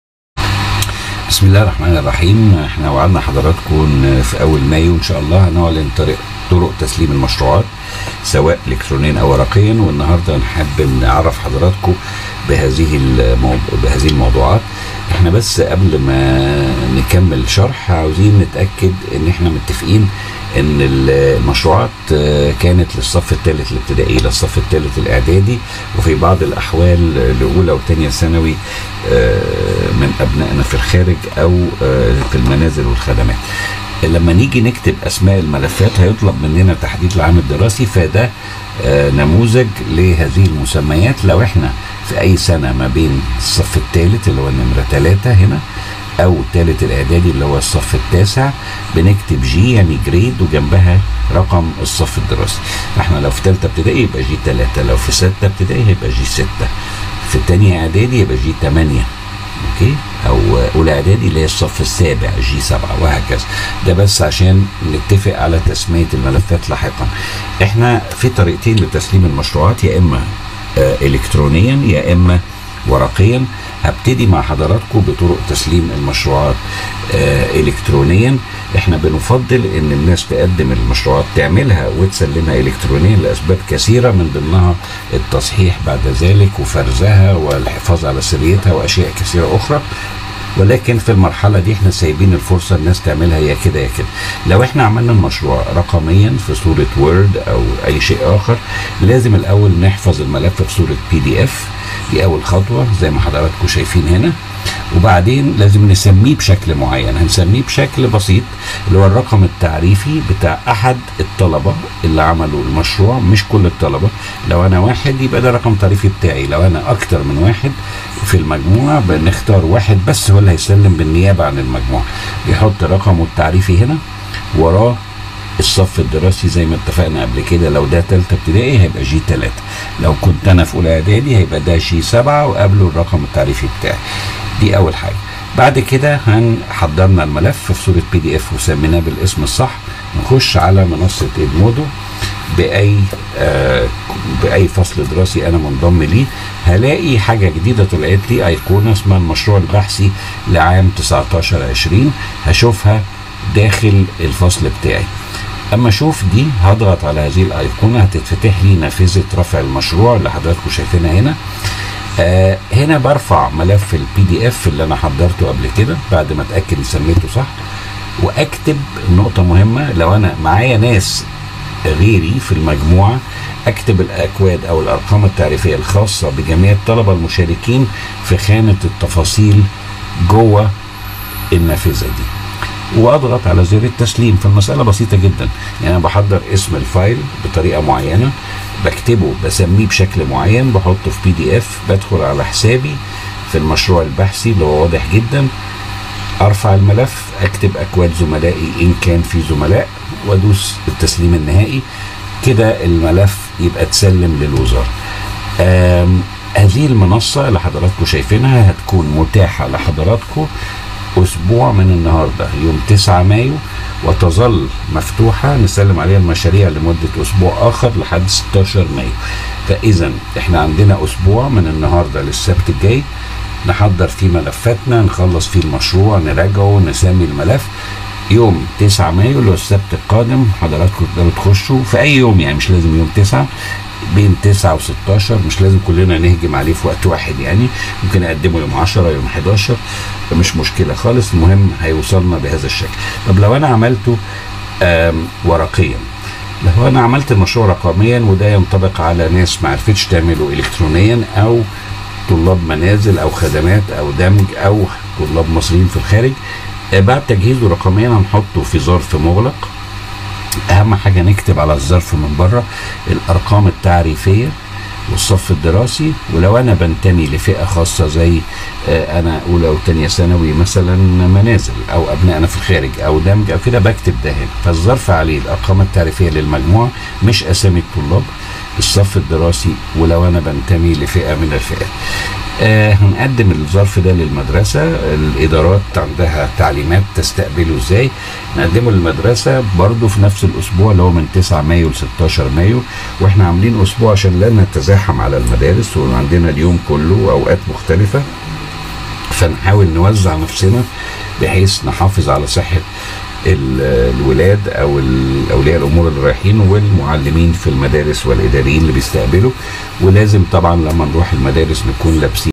كيفيه تسليم البحث علي المنصه edmodo اليكترونيا وورقيا
الوزير يحدد طريقه تسليم الابحاث علي المنصه
للتسجيل في المنصه ادمودو edmodo  اتبع الرابط التالي

بعض المصارد التي يمكنك الاستعانه بها للبحث الخاص بك
المكتبه الرقميه 👇 👇 👇 👇 👇 👇 👇 👇

وزير_التعليم يحدد كيفيه تسليم الابحاث علي المنصه ادمودو
مشروع البحث؛ كيفية عمل مشروعات البحث، بحث للصف الثاني الإعدادي، أبحاث الصف الثالث الإعدادي، أبحاث الصفوف المختلفه